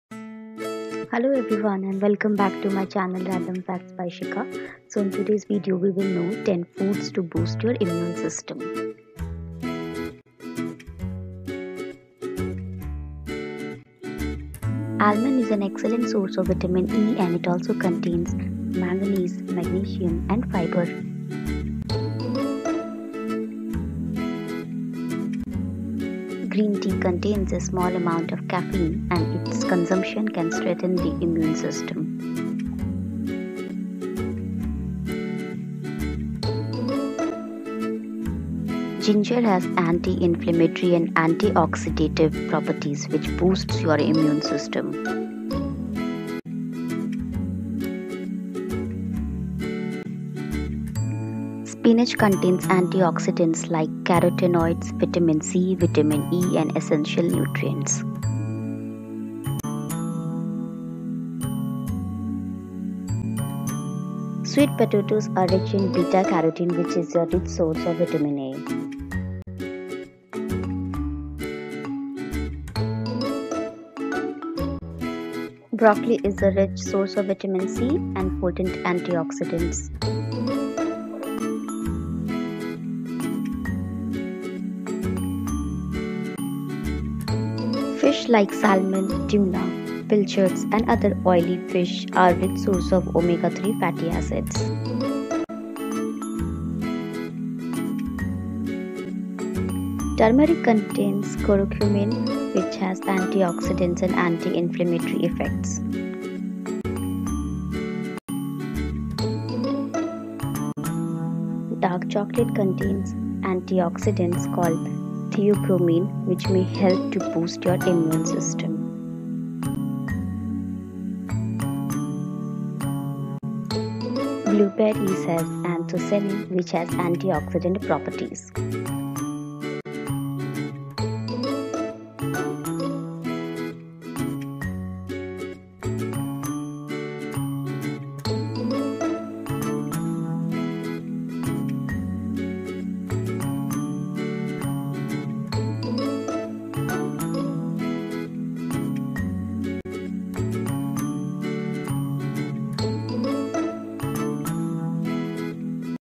10 Foods To Boost Your Immune System | इम्यून सिस्टम को मजबूत करे
Here is a list of 10 food that will help you to boost your immunity system.

Almond is an excellent source of vitamin E and it also contains manganese, magnesium, and fiber.
Green tea contains a small amount of caffeine and its consumption can strengthen the immune system.
Ginger has anti-inflammatory and anti-oxidative properties which boosts your immunity system.
Spinach contains antioxidants like Carotenoids, Vitamin C, Vitamin E and essential nutrients.
Sweet potatoes are rich in beta carotene which is a source of vitamin A.
Broccoli is a rich source of vitamin C and potent antioxidants.
Salmon, tuna, pilchards, and other oily fish are a rich source of omega-3 fatty acids.
Turmeric contains curcumin, which has antioxidant and anti-inflammatory effects.